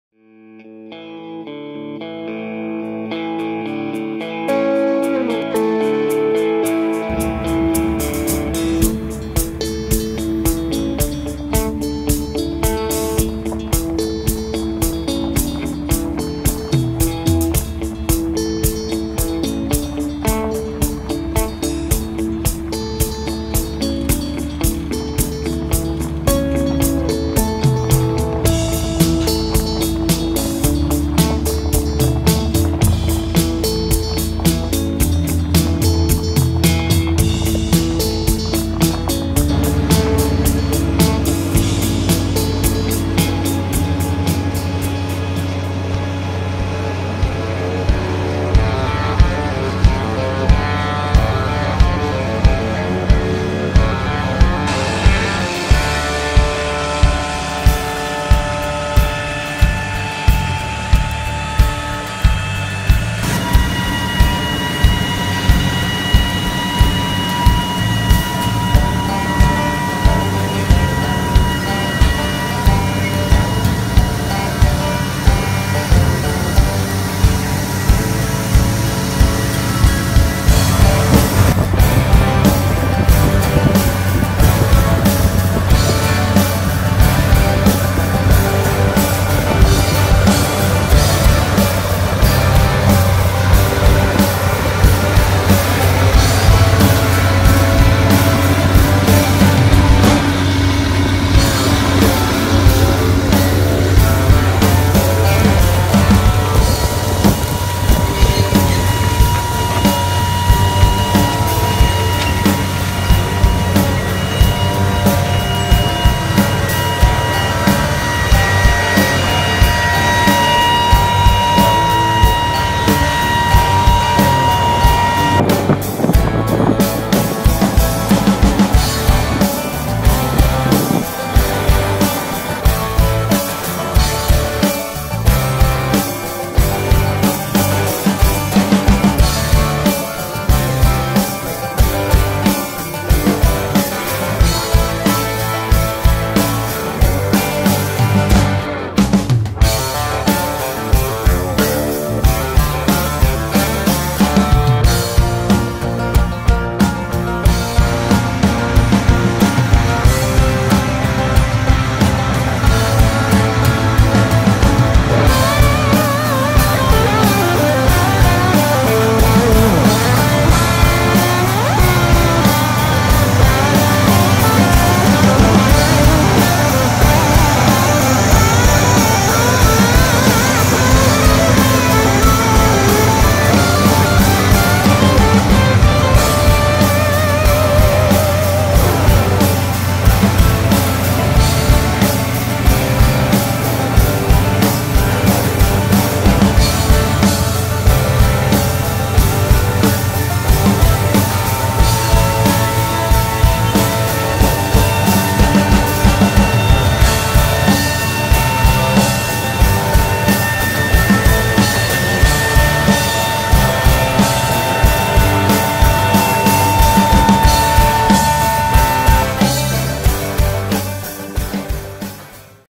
Seeding 2k16
New holland tractor and seeding seeding 4420 acres northern linterlake